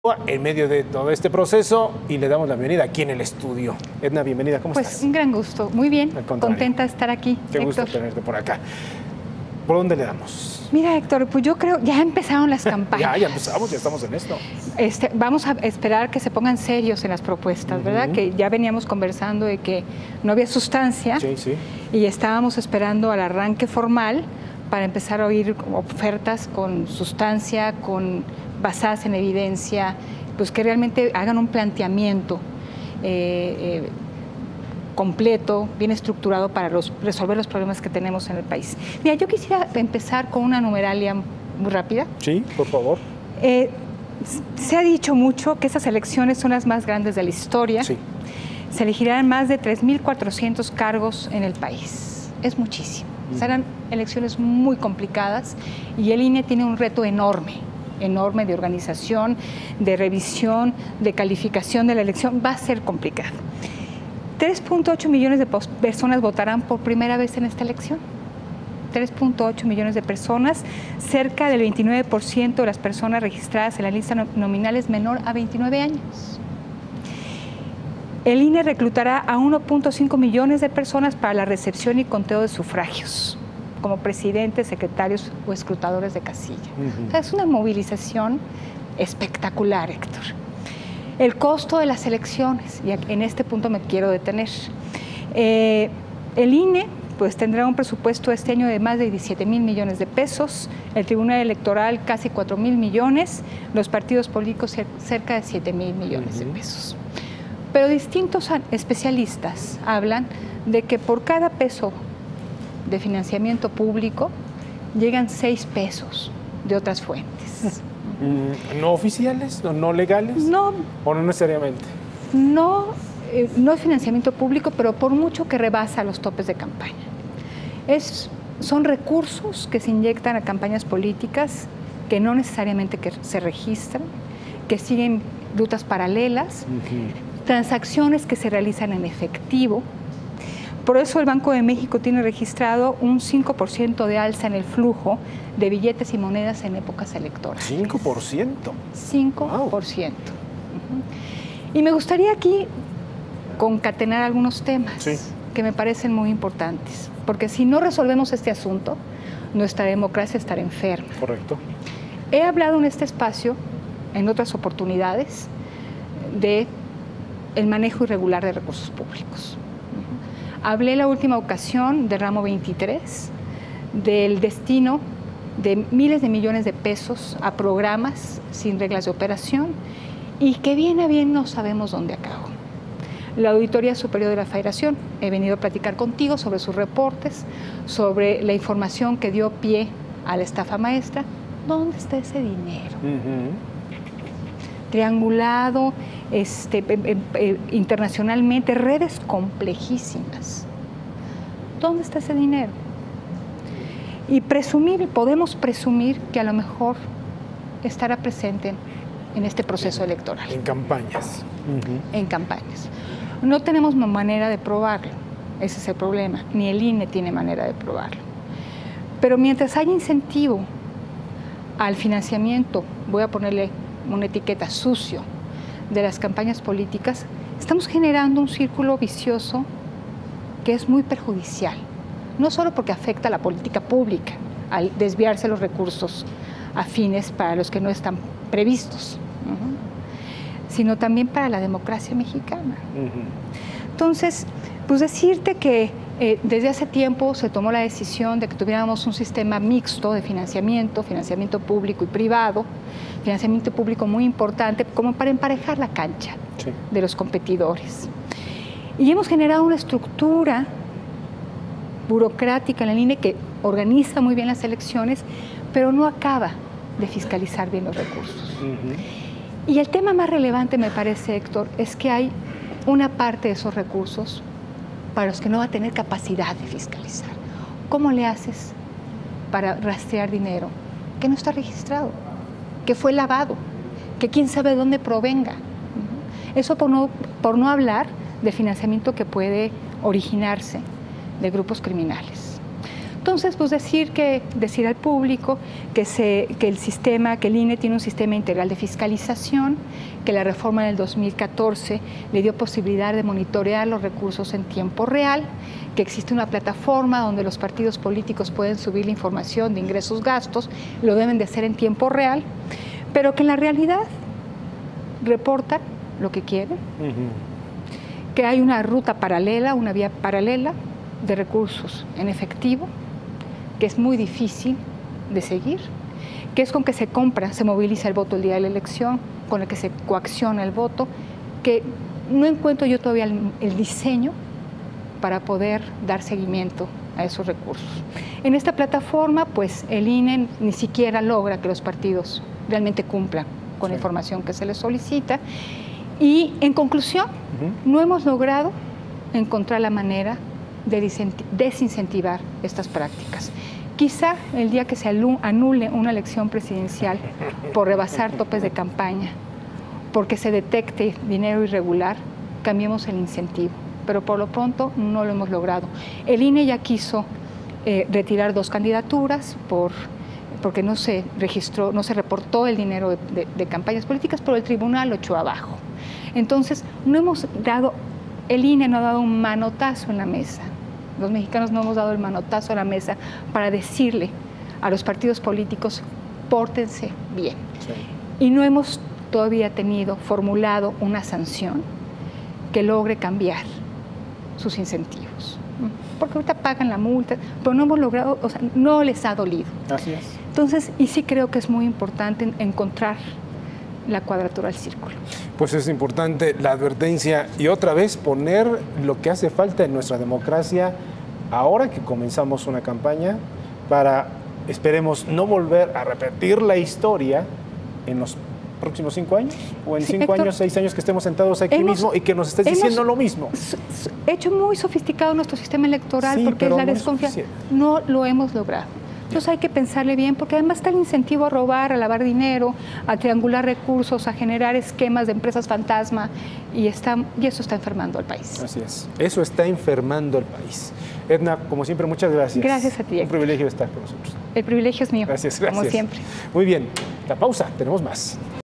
Autoridades no podrán fiscalizar todo los recursos en las campañas: Edna Jaime
Edna Jaime, directora general de México Evalúa. Tema: Gasto de campañas.